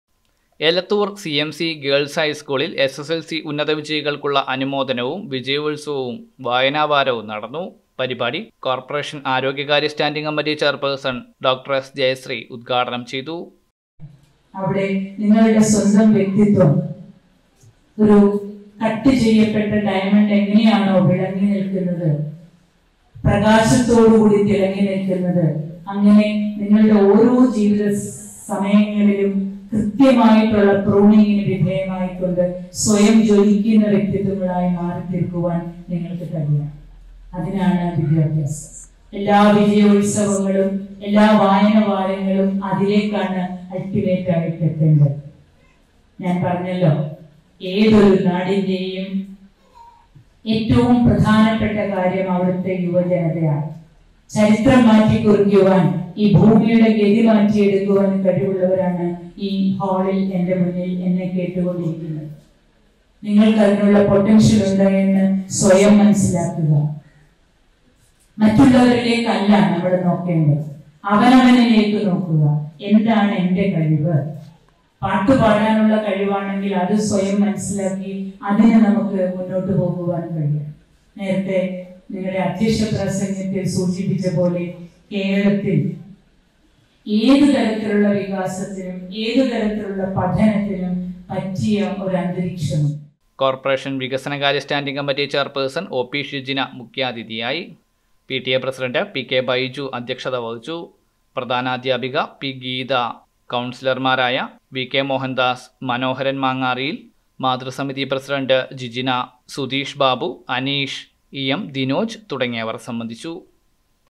എലത്തൂർ സിഎംസി ഗേൾസ് ഹൈസ്കൂളിൽ എസ്എസ്എൽസി ഉന്നത വിജയികൾക്കുള്ള അനുമോദനവും വിജയോത്സവവും വായനാവാരവും
എലത്തൂർ സിഎംസി ഗേൾസ് ഹൈസ്കൂളിൽ എസ്എസ്എൽസി ഉന്നത വിജയികൾക്കുള്ള അനുമോദനവും വിജയോത്സവവും വായനാവാരവും നടന്നു .കോർപ്പറേഷൻ സ്റ്റാൻഡിങ് കമ്മിറ്റി ചെയർപേഴ്സൺ ഡോക്ടർ എസ് ജയശ്രീ  ഉദ്ഘാടനം ചെയ്തു